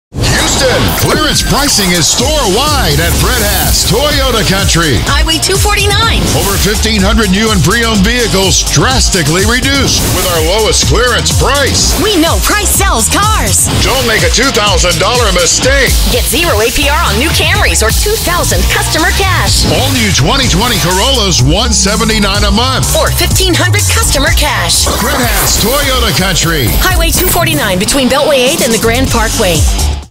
Fred Haas Toyota Country - National Clearance Event - Car
Houston! Clearance pricing is store wide at Fred Haas Toyota Country. Highway 249. Over 1500 new and pre-owned vehicles drastically reduced with our lowest clearance price. We know price sells cars. Don't make a $2000 Mistake. 0% APR on new Camrys or $2000 customer cash. All new 2020 Corollas $179 / month or $1500 customer cash. Fred Haas Toyota Country. Highway 249 between Beltway 8 and the Grand Parkway.